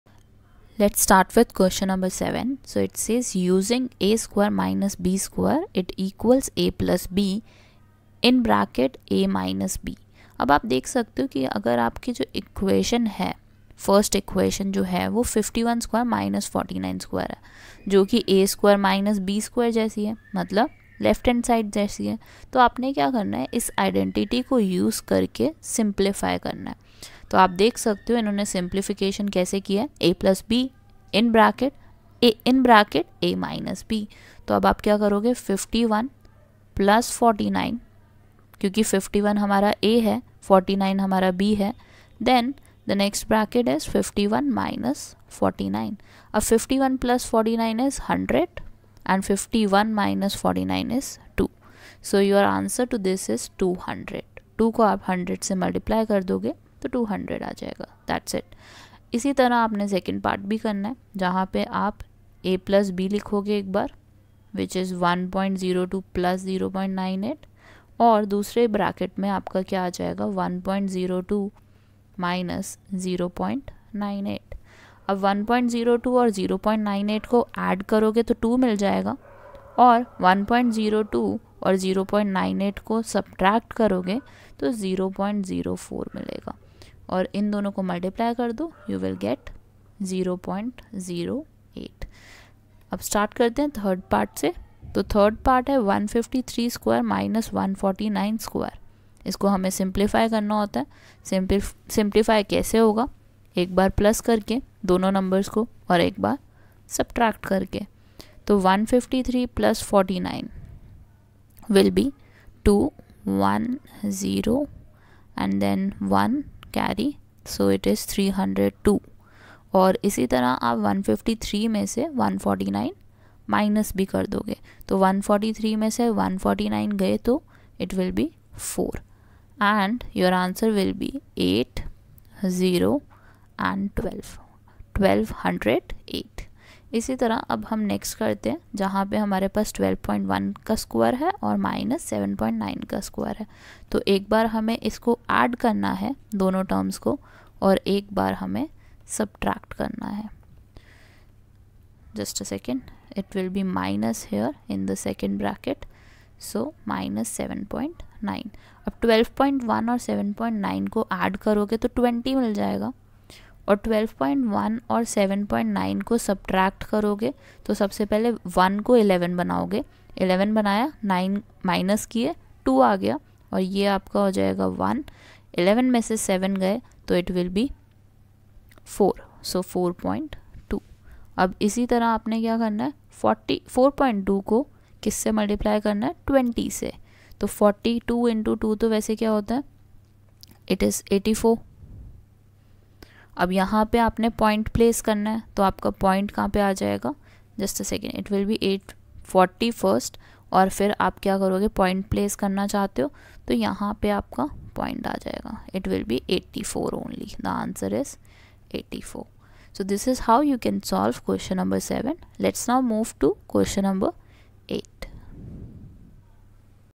7. Using a² – b² = (a + b) (a – b), find: 51² – 49², (1.02)² – (0.98)², 153² - 147², 12.1² - 7.9²
7. Using a² – b² = (a + b) (a – b), find:
51² – 49²,
(1.02)² – (0.98)²,
153² - 147²,
12.1² - 7.9²